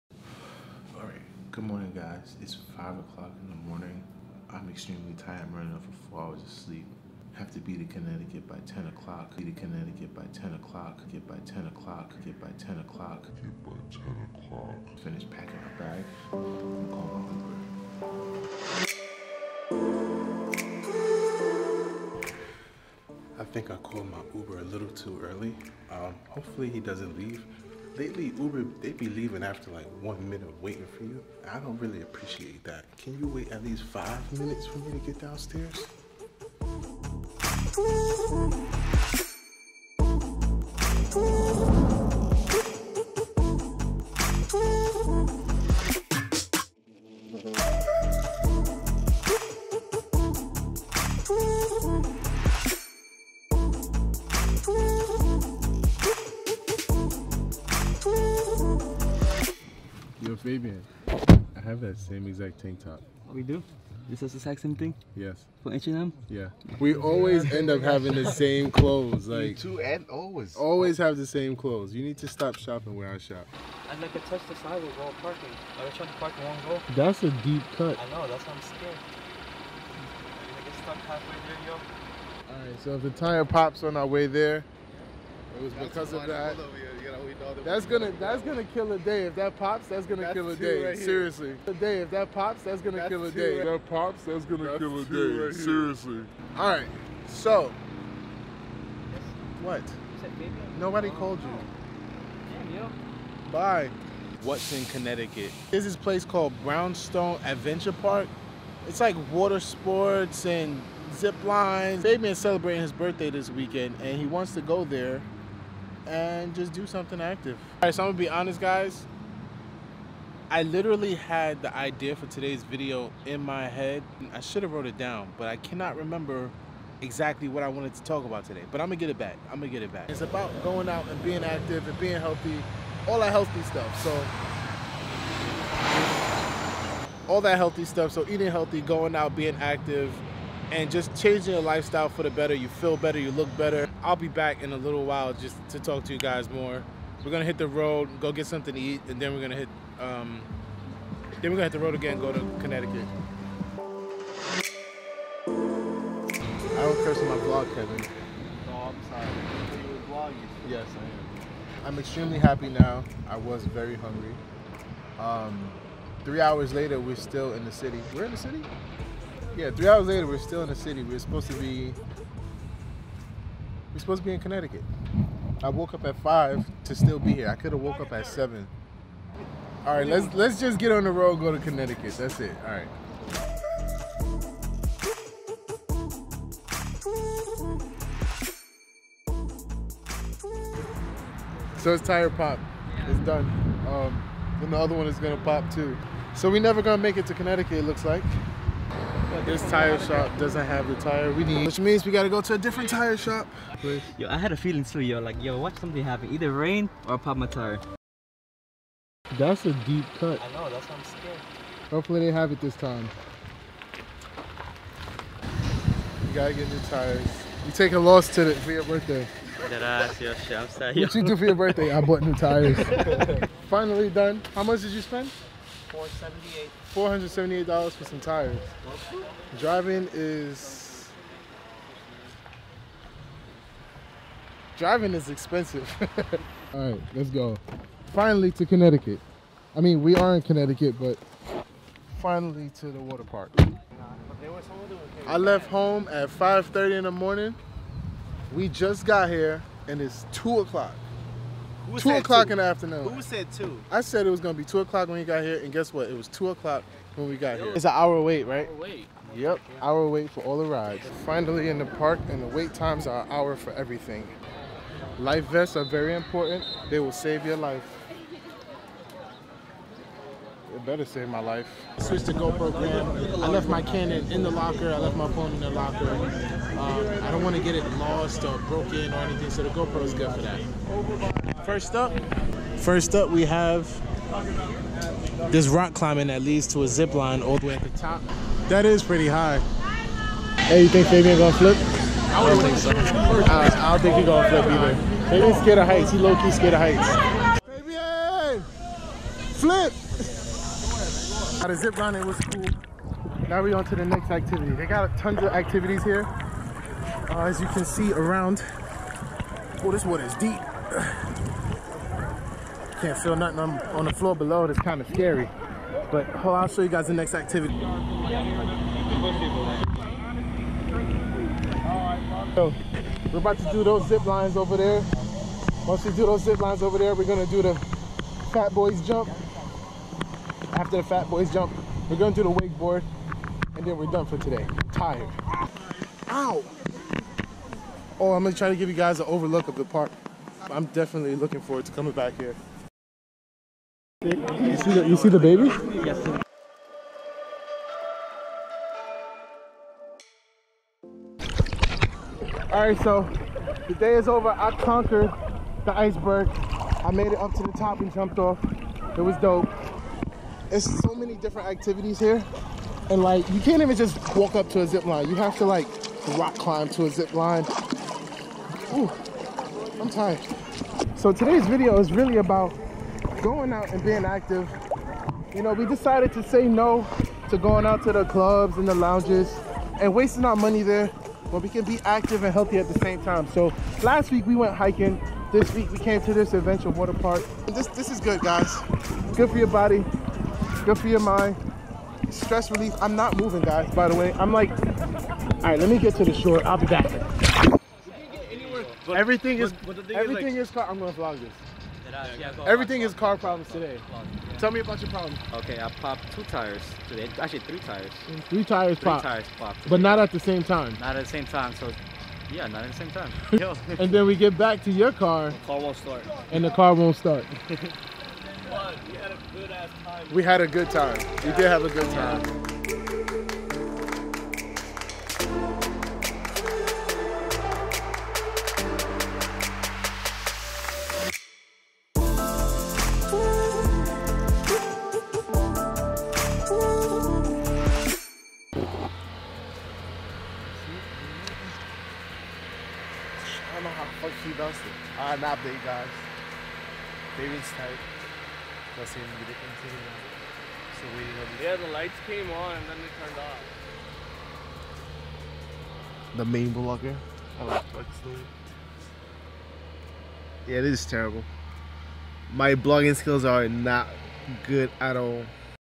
Change your Lifestyle! Brownstone Adventure Park
Change up your lifestyle by quitting the super lit, super expensive clubs and lounges and go explore and be active.

Last week was my first time hiking and this week was Brownstone Adventure Park

Shout out to my GoPro for taking on half of the vlog today.